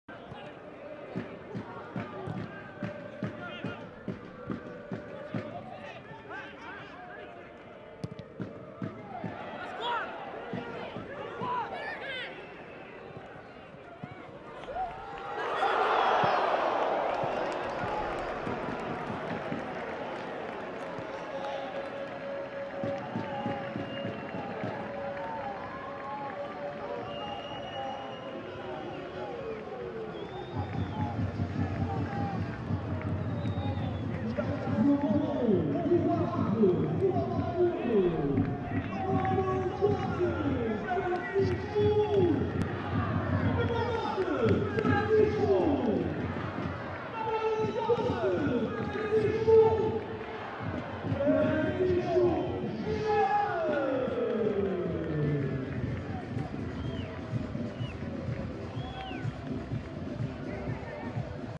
Golo de Francisco Geraldes: Rio Ave (2)-0 Estoril (Liga 24ªJ)
Subscreve o nosso canal e ativa as notificações para receberes os alertas dos golos em tempo real! 🔔